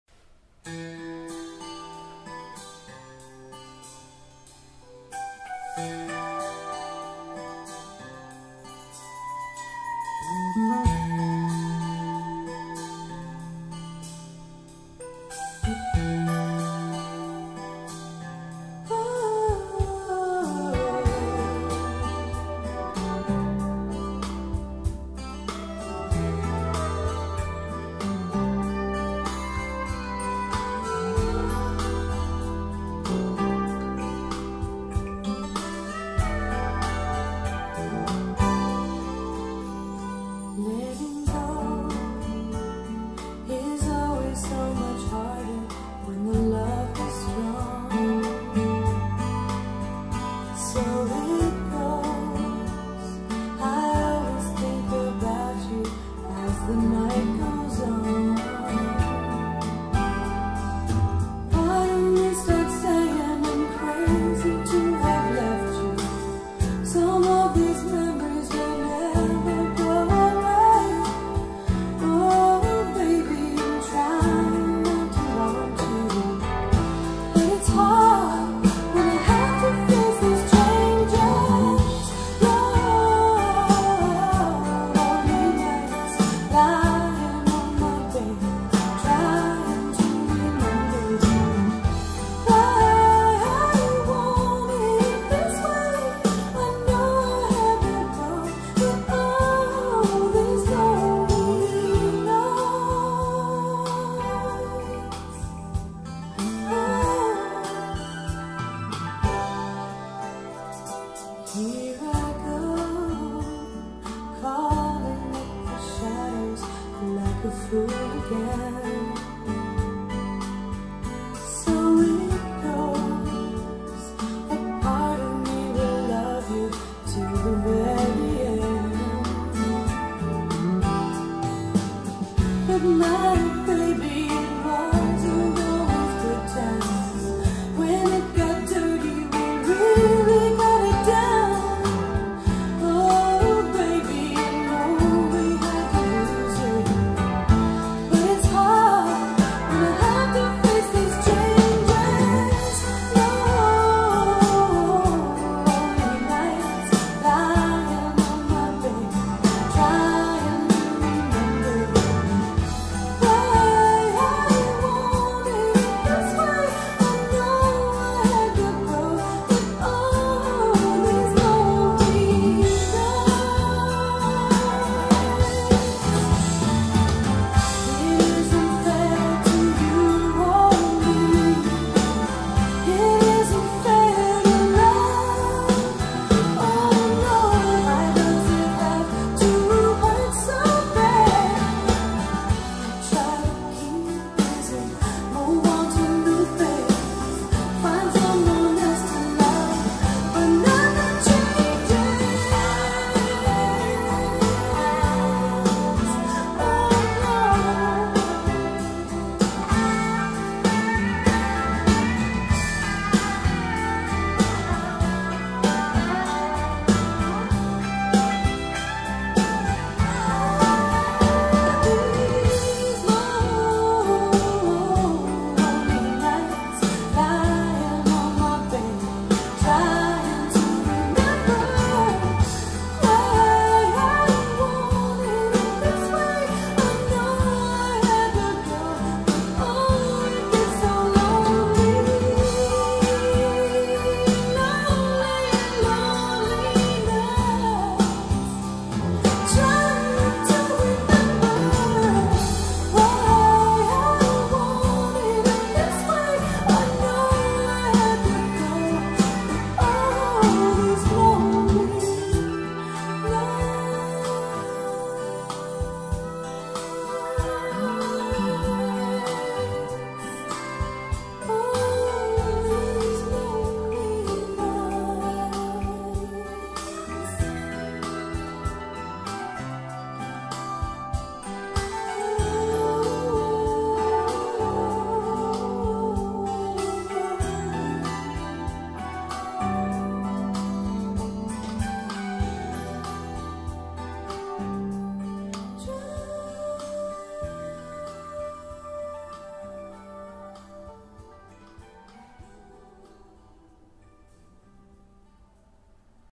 Focal Kanta2 #1 (before)
amp + dac, + player = Naim Uniti Star,
record = sony pcm d100

랜케이블을 보유하고 계신신것중 좋아보이는 번들로 교체, 공유기, 셋탑박스 순서로 극성을 맞췄습니다.